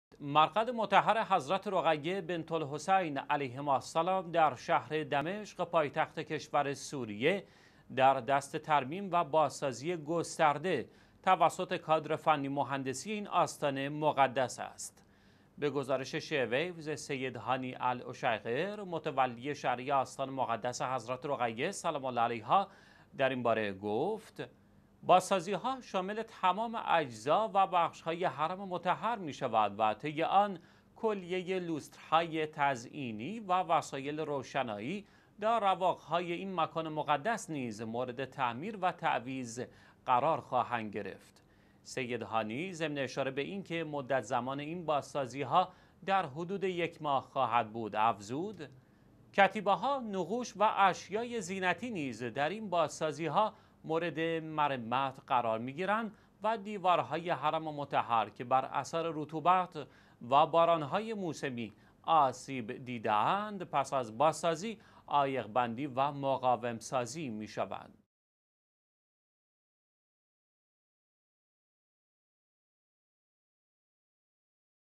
بازسازی قسمت هایی از حرم مطهر حضرت رقیه سلام الله علیها در سوریه
مرقد مطهر حضرت_رقیه بنت الحسین علیهما السلام در شهر دمشق پایتخت کشور سوریه، در حال ترمیم و بازسازی گسترده از سوی کادر فنی-مهندسی این آستان مقدس می باشد.
به گزارش شیعه ویوز، سید هانی الاشیقر متولی شرعی آستان مقدس حضرت رقیه سلام الله علیها در این باره گفت: این بازسازی ها، شامل تمامی اجزا و بخش های حرم_مطهر می باشد که در آن کلیه لوستر های تزئینی و و سایل روشنایی در رواق های این مکان مقدس مورد تعمیر و تعویض قرار خواند گرفت.
وی ضمن اشاره به اینکه مدت زمان این بازسازی ها در حدود یک ماه می باشد، افزود: کتیبه ها، نقوش و اشیای زینتی نیز در این بازسازی ها مورد مرمت قرار گرفته و با هدف مقاوم سازی دیوارهای حرم مطهر که بر اثر رطوبت و باران های موسمی آسیب دیده اند، پس از بازسازی عایق بندی گشته و مقاوم سازی می شوند.

سه شنبه - ۱ خرداد ۱۳۹۷
الثلاثاء - ٦ رمضان ١٤٣٩